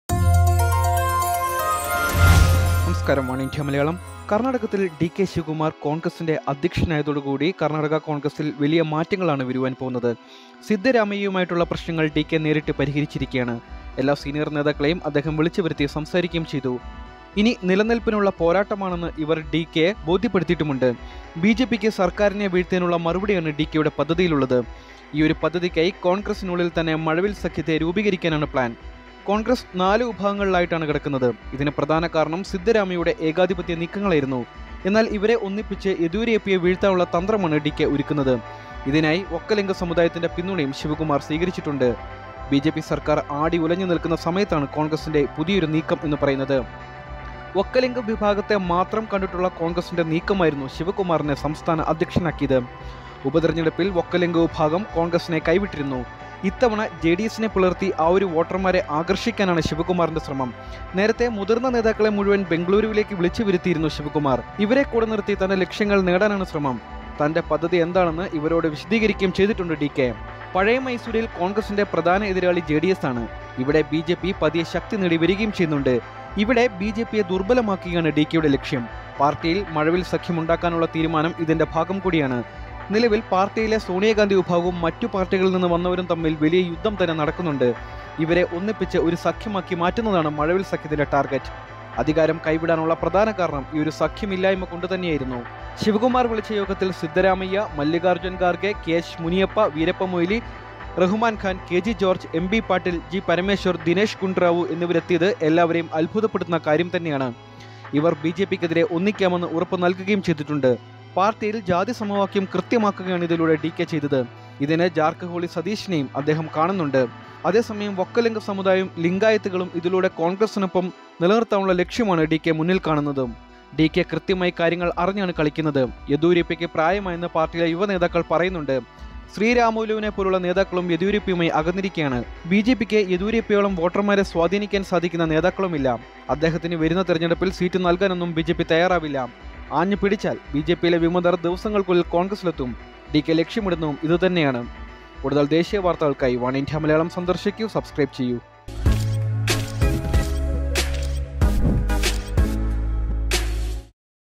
Big Task Ahead For DK Shivakumar | Oneindia Malayalam
Big Task Ahead For DK Shivakumar
കര്‍ണാടകത്തില്‍ ഡികെ ശിവകുമാര്‍ അധ്യക്ഷനായതോടെ മാറ്റങ്ങള്‍ ഒരുങ്ങുന്നു. സിദ്ധരാമയ്യയുമായുള്ള പ്രശ്‌നങ്ങള്‍ ഡികെ നേരിട്ട് പരിഹരിച്ചിരിക്കുകയാണ്.